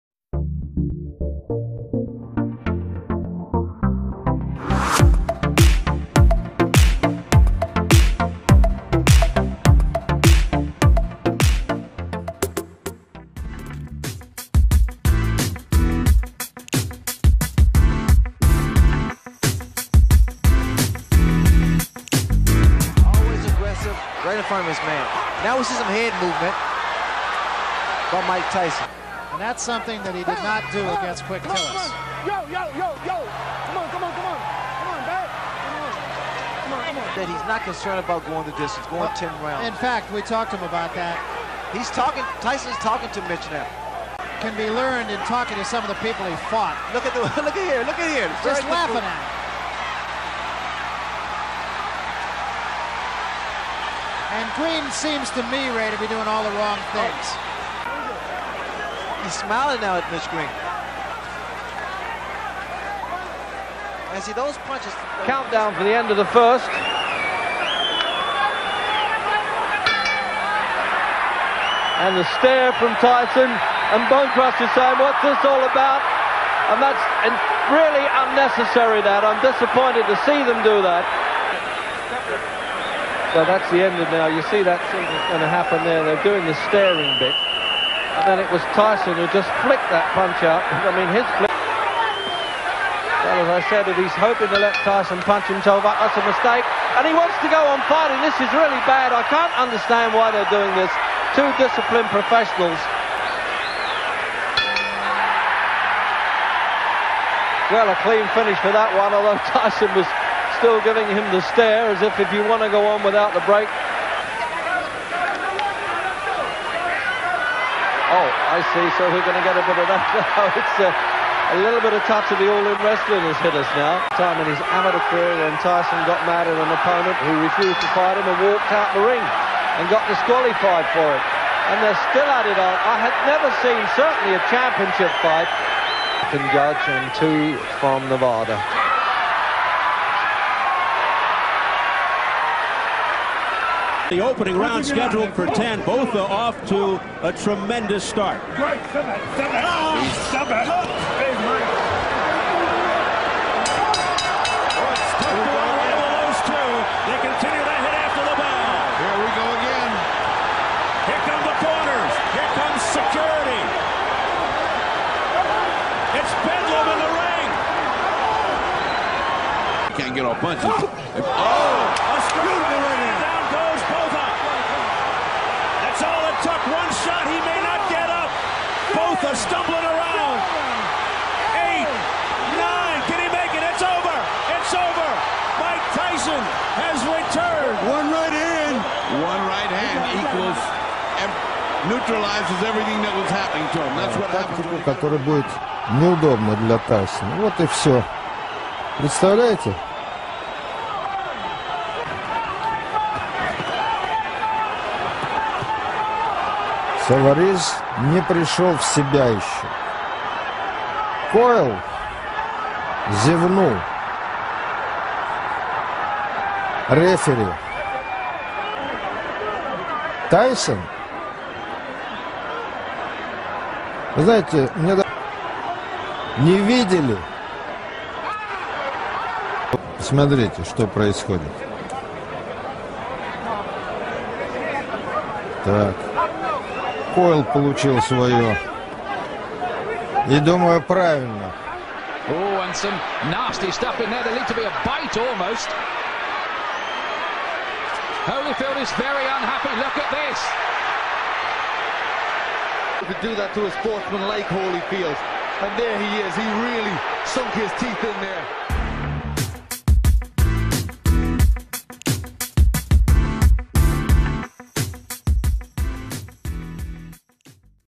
TOP 5 MOST INSANE FIGHTS with MIKE TYSON !!!
The BEST TOP 5 insane fights that MIKE TYSON have been with some if the greatest fighter such as Mitch Green, James Smith, Francois Botha, Lou Savarese and Evander Holyfield.